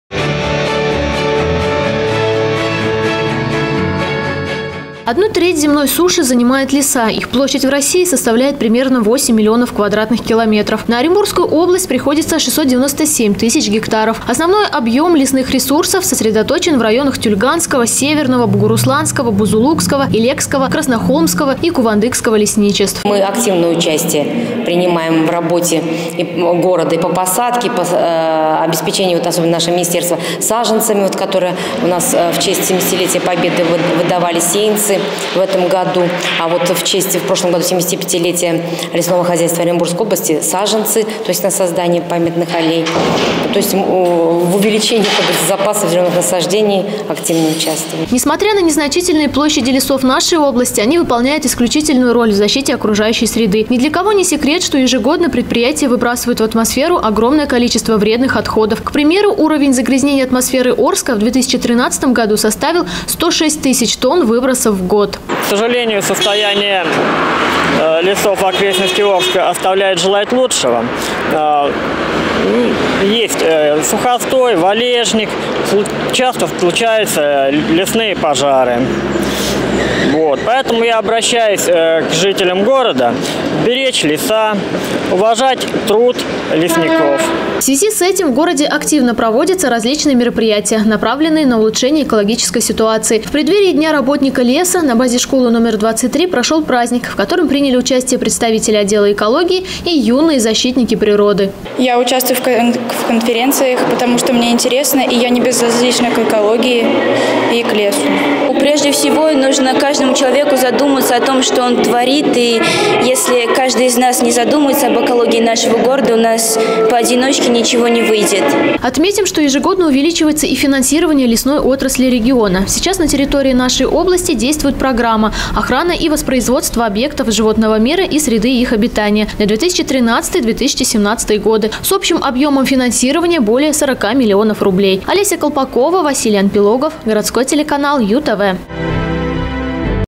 UTV. День работников леса. г.Орск
Каждое третье воскресенье сентября в России отмечается День работников леса.Это праздник людей, жизнь которых связана с охраной и приумножением лесных богатств нашей страны,особенно в наше сложное с экологической точки зрения время. Как обстоят дела с лесным хозяйство в нашей области,в материале Олеси Колпаковой.